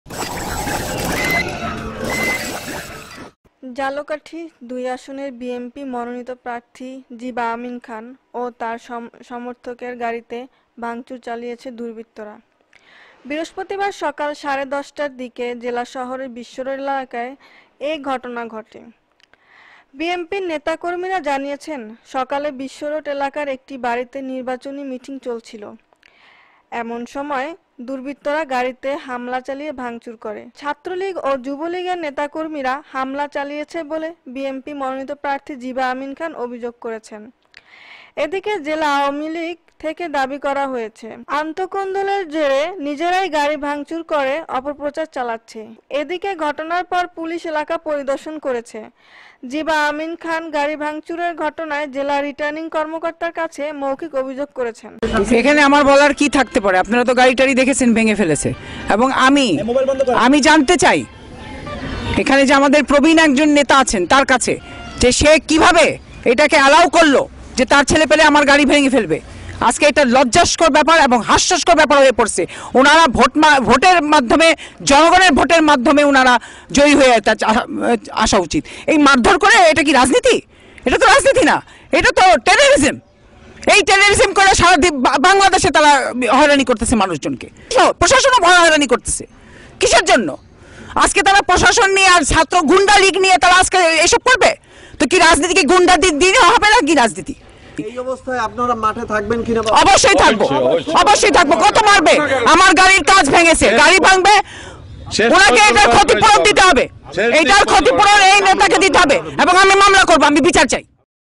ঝালকাঠি-২ আসনের বিএনপি প্রার্থী’র নির্বাচনী মিটিংয়ে ভাংচুর
ঝালকাঠি-২ আসনের বিএনপি প্রার্থী নির্বাচনী মিটিংয়ে সমর্থকের গাড়িতে ভাংচুর
ঝালকাঠি প্রতিনিধি: ঝালকাঠি-২ আসনের বিএনপি মনোনিত প্রার্থী জীবা আমিনা খান ও তার সমর্থকের গাড়িতে ভাংচুর চালিয়েছে দুর্বৃত্তরা। আজ বৃহস্পতিবার সকাল সাড়ে ১০টার দিকে জেলা শহরের বিশ্বরোড এলাকায় এ ঘটনা ঘটে।
বিএনপির নেতা কর্মীরা জানিয়েছে, সকালে বিশ্বরোড এলাকার একটি বাড়িতে নির্বাচনী মিটিং চলছিলো। এসময় দুর্বৃত্তরা গাড়িতে হামলা চালিয়ে ভাংচুর করে। ছাত্র-লীগ ও যুবলীগের নেতাকর্মীরা হামলা চালিয়েছে বলে  বিএনপি মনোনিত প্রার্থী জীবা আমিনা খান অভিযোগ করেছেন।
এদিকে জেলা আওয়ামী লীগ থেকে দাবী করা হয়েছে, আন্তকোন্দলের জেরে নিজেরাই গাড়ি ভাংচুর করে অপপ্রচার চালাচ্ছে।
এদিকে ঘটনার পর পুলিশ এলাকা পরির্দশ করেছে। জীবা আমিন খান গাড়ি ভাংচুরের ঘটনায় জেলা রির্টানিং কর্মকর্তার কাছে মৌখিক অভিযোগ করছেন।